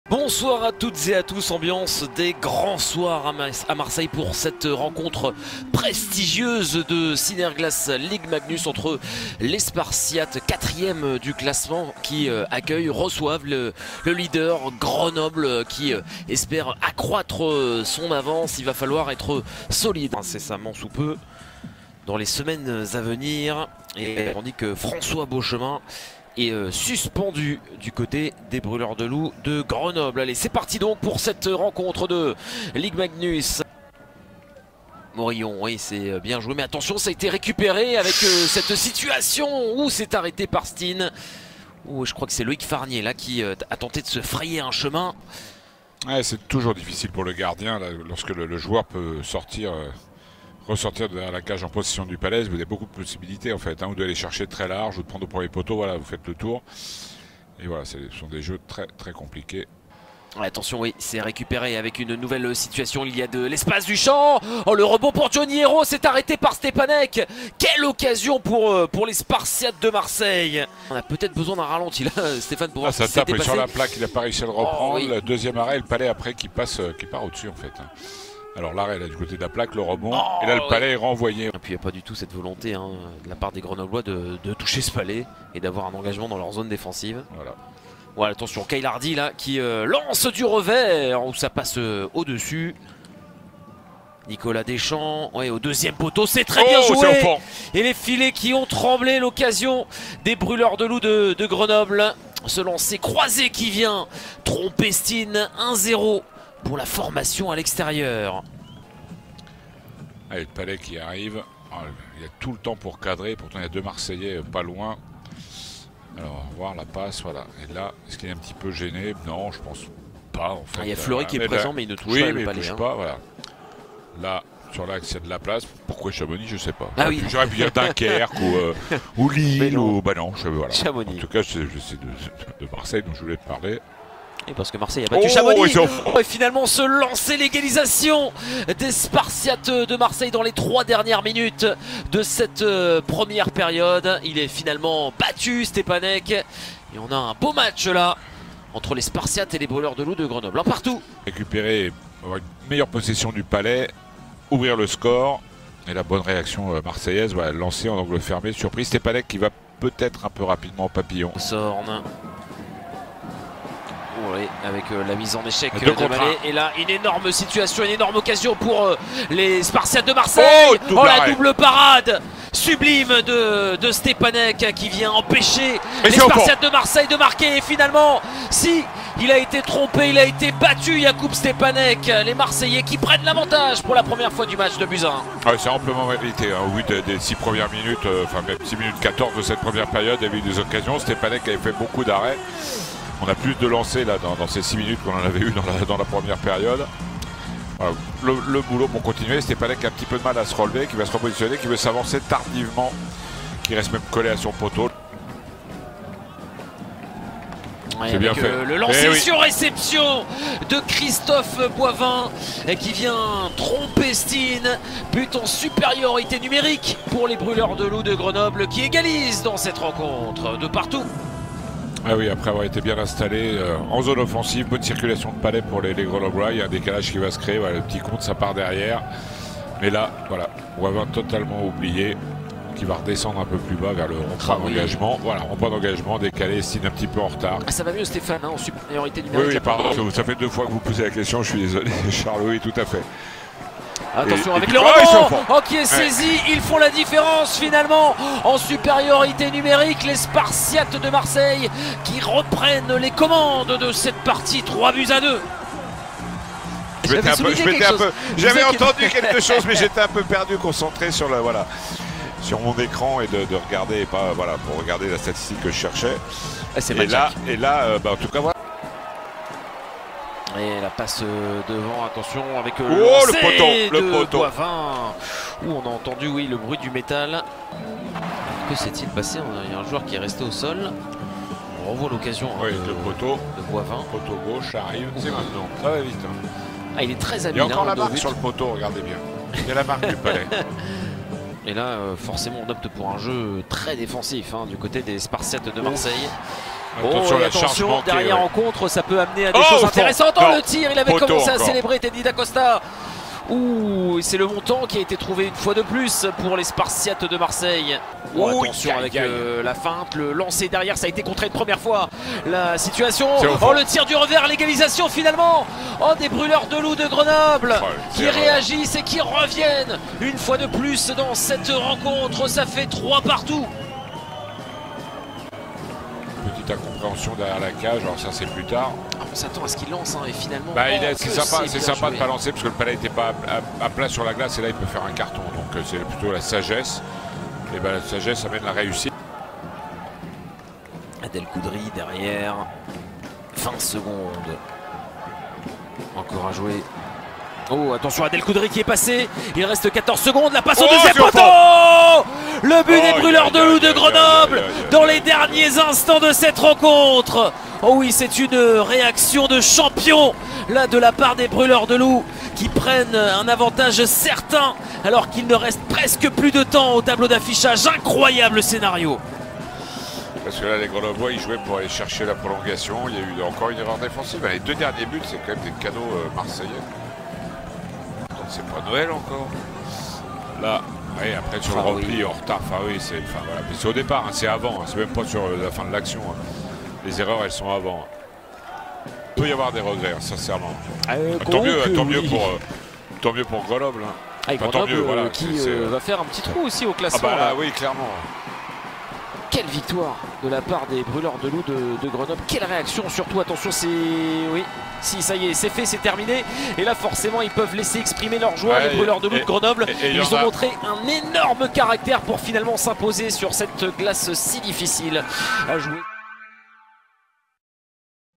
🏒 Le résumé de MARSEILLE - GRENOBLE (J17) | Synerglace Ligue Magnus 2024/25
Le résumé de la 17ème journée de Synerglace Ligue Magnus entre Marseille et Grenoble.